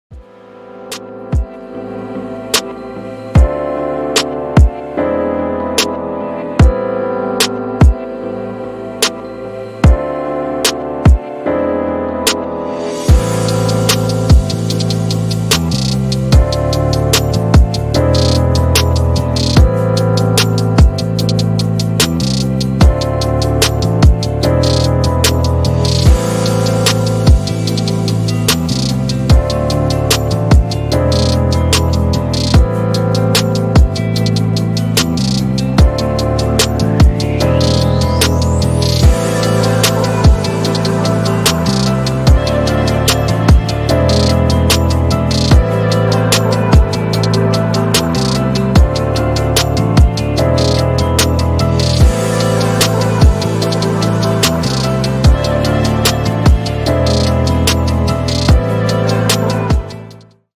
Termix en el backstage de los premios Goya | by Lorena Morlote
Un año más, Termix vuelve a estar presente en el backstage de la mano de la estilista Lorena Morlote, creando y retocando los peinados de las actrices y actores que han entregado los premios.
Descubre los looks tendencia que hemos visto este fin de semana en la ceremonia de los premios Goya 2022 de la Academia del Cine español.